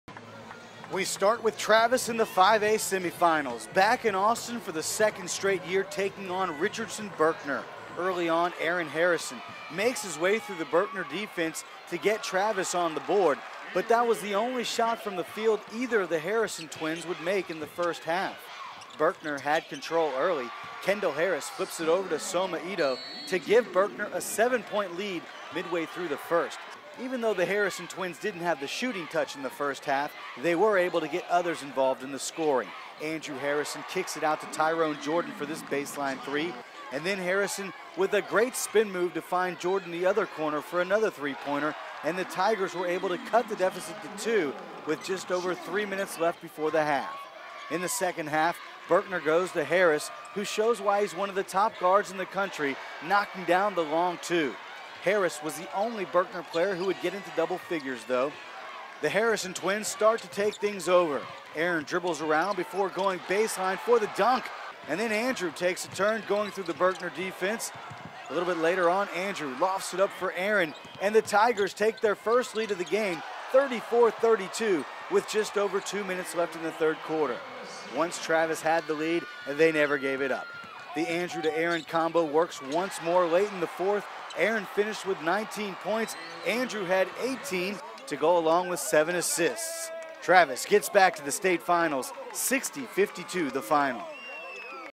Travis Tigers vs. Richardson Berkner Rams - 2013 Basketball - Week 10
We start with Travis in the 5A semifinals, back in Austin for the 2nd straight year taking on Richardson Berkner. Early on Aaron Harrison makes his way through the Berkner defense to get Travis on the board, but that was the only shot from the field either of the Harrison twins would make in the first half. Berkner had control early, Kendal Harris flips it over to Soma Edo to give Berkner a 7 point lead midway thru the first. Edo will be playing for Fresno State next season. Travis doesn't go away, even though the Harrison twins didn't have the shooting touch in the first half, they were able to get others involved in the scoring, Andrew Harrison kicks it out to Tyronne Jordan for this baseline 3 pointer in the 2nd quarter and then Harrison with a great spin move to find Jordan in the other corner for another three pointer, and the Tigers were able to cut the deficit to 2 with just over 3 left before the half. 2nd half Berkner goes to Kendal Harris who shows why he's one of the top guards in the country knocking down the long 2, Harris was the only Berkner player who would get into double figures though. The Harrison twins start to take things over, Aaron dribbles around before going baseline for the dunk. Then Andrew takes a turn going through the Berkner defense. Andrew lofts it up for Aaron and the Tigers take their first lead of the game 34-32 with just over 2 minutes left in the 3rd. Once Travis had the lead, they never gave it up, the Andrew to Aaron combo works once more late in the fourth Aaron finished with 19 points, Andrew had 18 to go along with 7 assists as Travis gets back to the state finals 60-52.